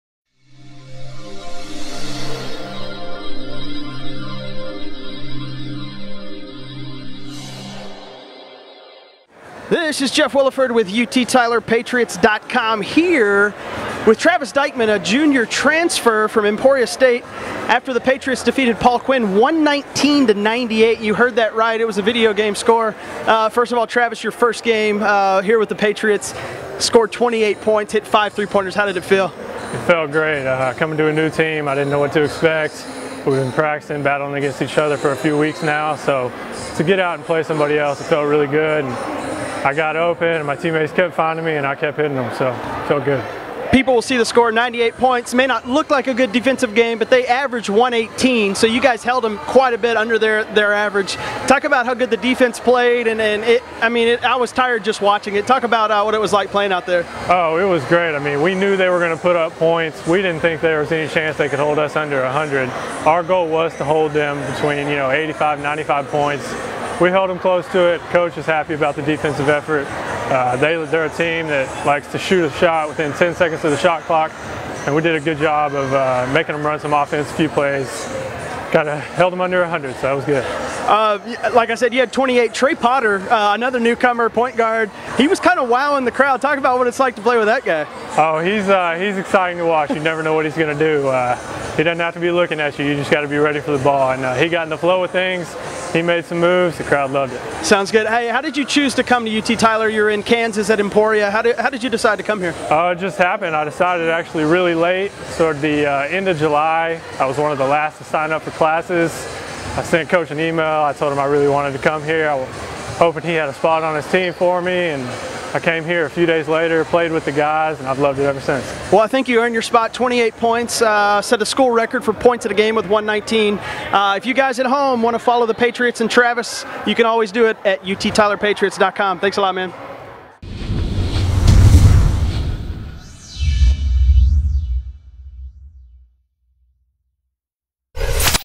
Travis Dykman - UT Tyler Men's Basketball (Nov. 17, 2012)
TYLER -- The UT Tyler men's basketball team won its season opener in historic fashion Saturday night, defeating Paul Quinn 119-98 at the Herrington Patriot Center.

The 119 points were the most ever scored in a game for the Patriots, breaking the previous mark of 112 set during a loss to Concordia in 2006.

The game started in bizarre-fashion, with the Patriots (1-0) awarded two free throws after a technical foul was given to the Tigers (5-1) for illegal uniform numbers.

With a 1-0 lead before the tipoff, UT Tyler took advantage of the momentum to go up 19-7 in the first seven minutes.

The Patriots extended their lead to 37-18 with 7:20 remaining, taking their largest lead of the first half.

Paul Quinn was able to cut into the UT Tyler-lead, going on a short 10-4 run to cut the  lead to 42-34 UT Tyler.

The Patriots finished the first half with a 18-11 run of their own, highlighted by a nice inbounds play to Stephan Tarpley with just three seconds remaining, closing the first half with a 60-45 lead.

Newcomer Travis Dykman was huge in the first half, hitting four three-pointers in clutch situations, leading to 18 first-half points.

The second half was much of the same for both teams, with the Tigers playing full-court, uptempo basketball that opened lanes for the Patriots.

Dykman hit his fifth-three of the night with 11:00 minutes remaining, making the score 86-70 and the momentum all on the side of the home team.

The only remaining question for the evening was would the Patriots break the 100-point mark and with 7:23 remaning in the game, UT Tyler took a 101-80 lead after a layup by junior Tre Potter, who scored 10 on the night.

Paul Quinn would not get any closer the rest of the way, falling 119-98.

Dykman led all-scorers with 28 total points to go along with seven rebounds while senior Darren Houliston added 24 points and seven rebounds.

Jordan Cannon added 13 points while teammates Nathan Harbour and Potter finished with 16 and 10 respectively.

The Patriots shot a staggering 60-percent from the field, and out-rebounded the Tigers 38-26.

UT Tyler will now head west to take-on American Southwest Conference opponent Howard Payne at 3:00 p.m. on Saturday, Nov. 24.